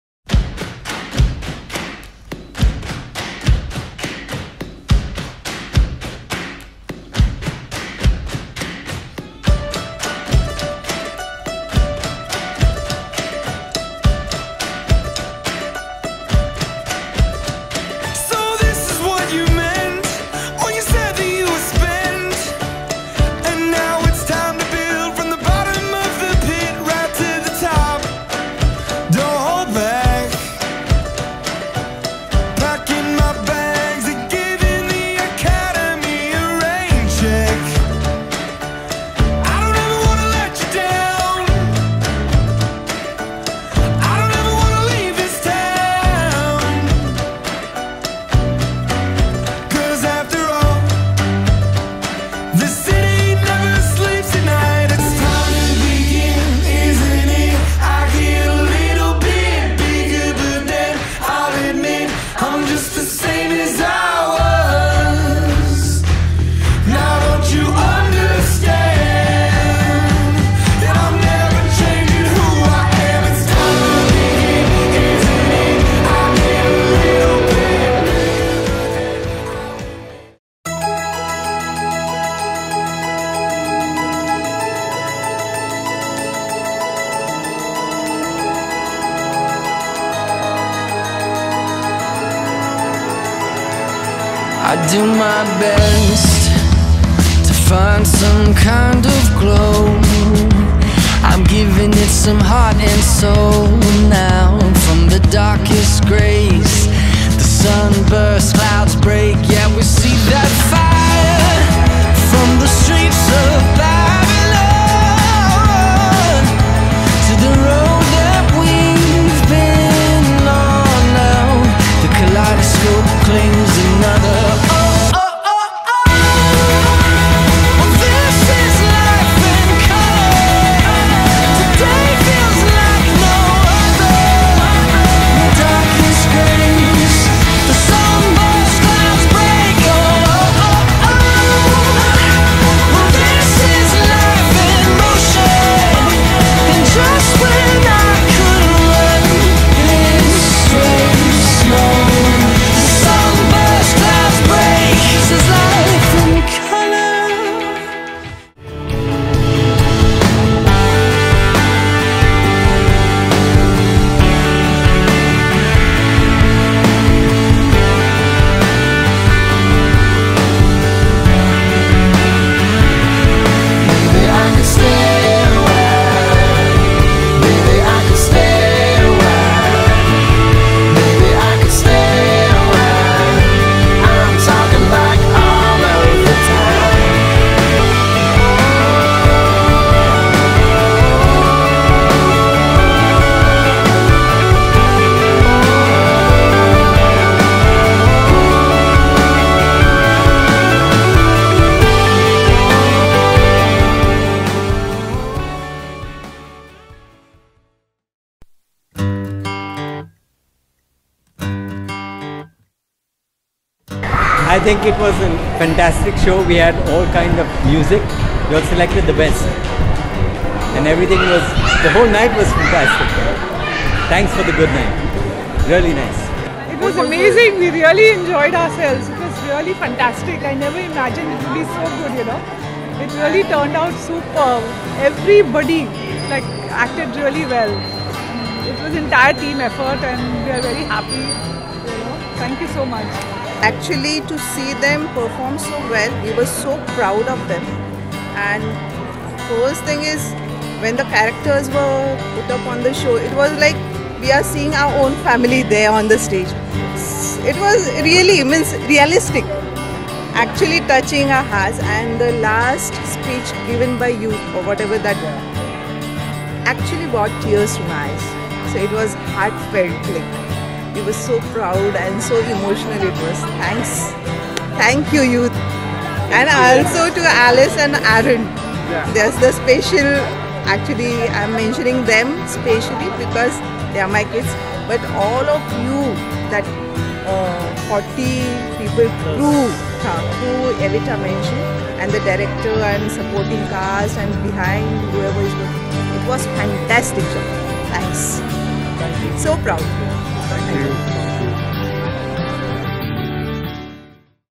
Those were the Days - A Recap Film ft. YOUTHopia
As we close the curtains on our wonderful show "Those were the Days" that brought laughter, romance, tears and happiness, here is our Recap Film from our show 🎥

Made by - Jason D'souza
Camera Ops - Jason D'souza, Simon Kerketta and Dylan D'silva